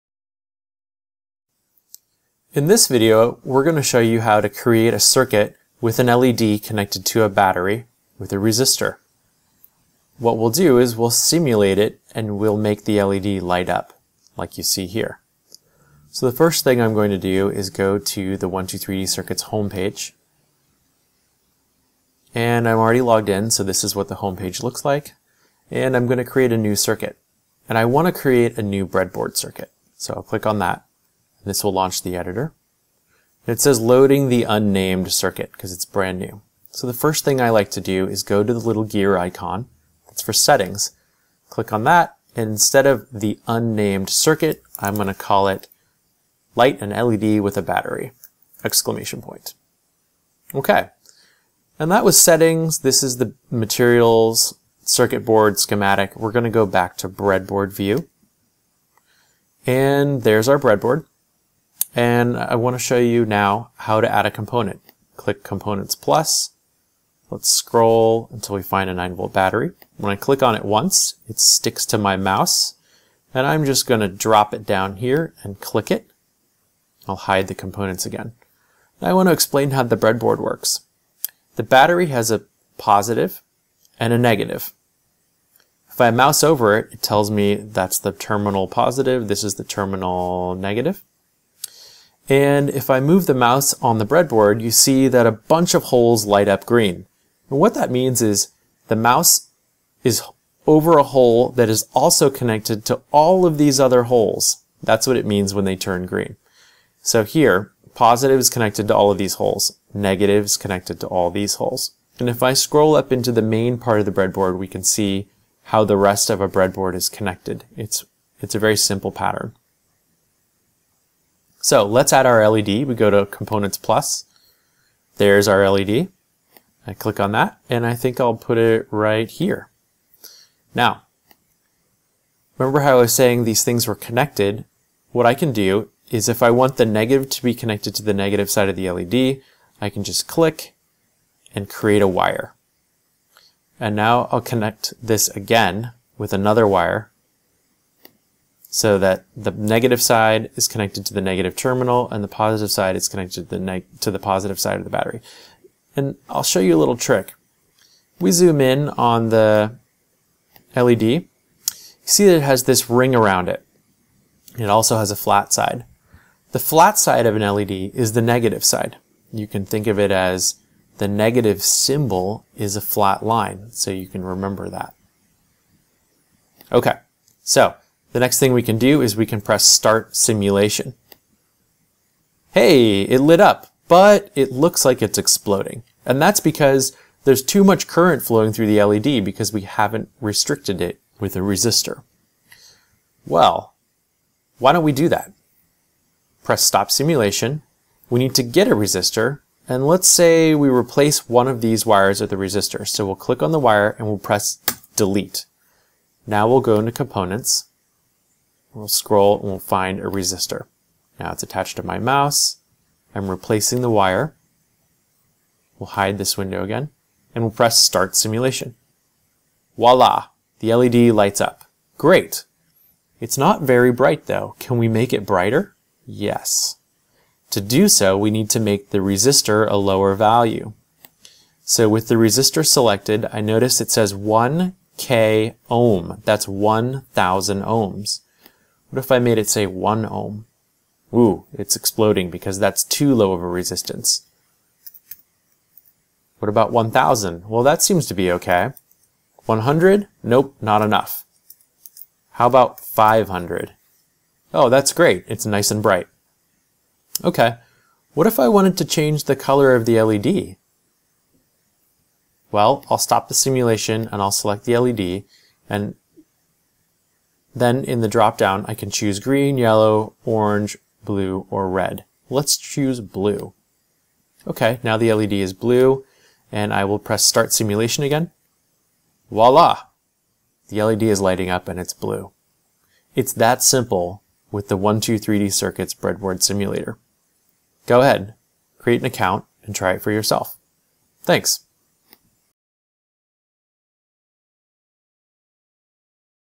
How To Light An LED - Electronics Lab
Watch this 5 minute video that shows how to connect a battery, resistor, and LED to make it light up.  It also shows how to use a breadboard.